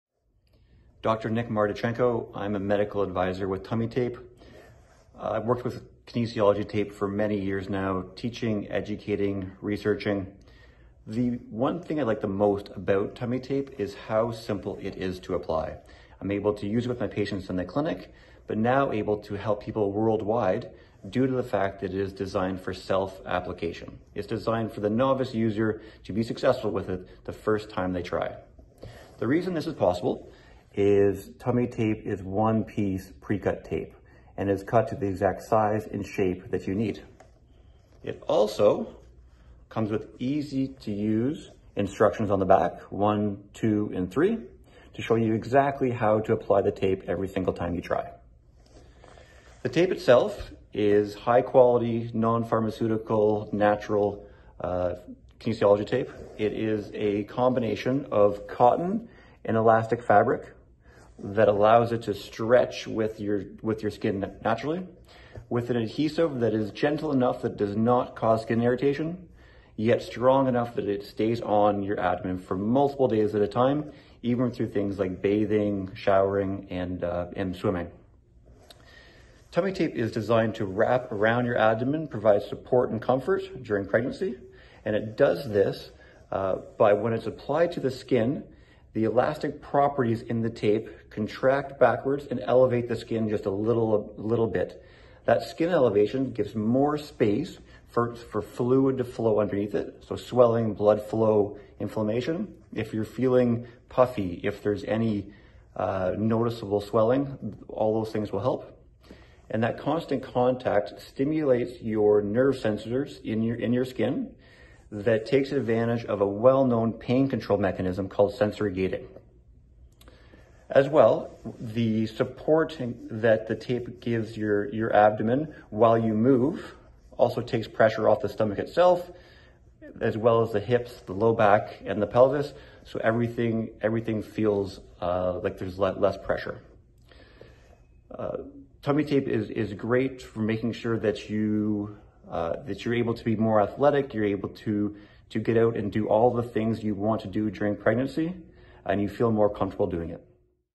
How Does Pregnancy Tape Work? | Learn More with Dr. Nick! | tummytape
Learn about the many benefits of using tummytape from medical advisor Dr. Nick Martichenko! Discover how our unique design provides all-natural comfort for our glowing moms-to-be, and how tummytape can relieve your pregnancy pains.

Let us lift you up!

tummytape provides a comfortable, safe, drug-free alternative to help ease pain during pregnancy. Relieve common pregnancy and maternity aches including back discomfort, swollen feet, sciatica, and more.